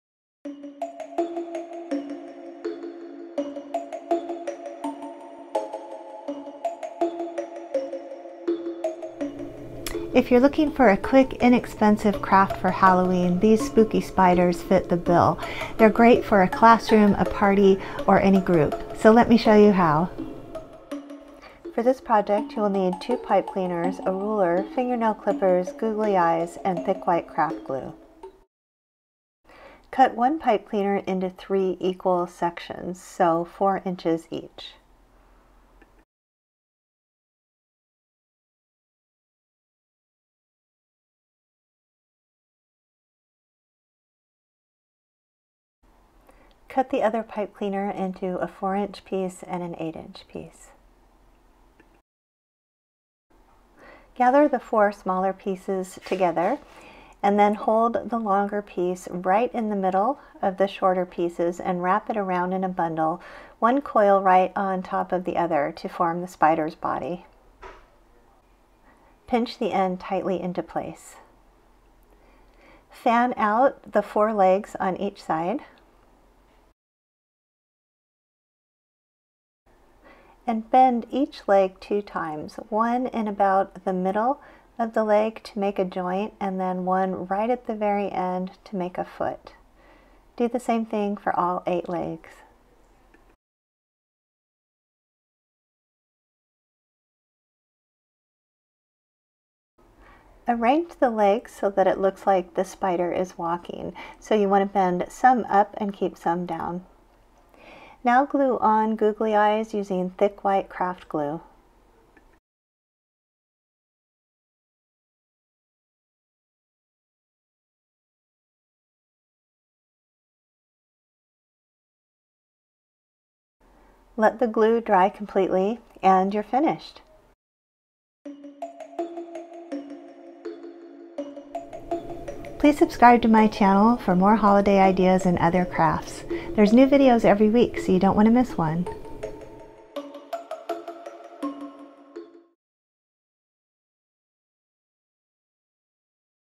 Pipe Cleaner Spiders - Small
Craft a host of spooky Halloween spiders from pipe cleaners. Great Halloween party activity or decorations. Easy, step-by-step instructions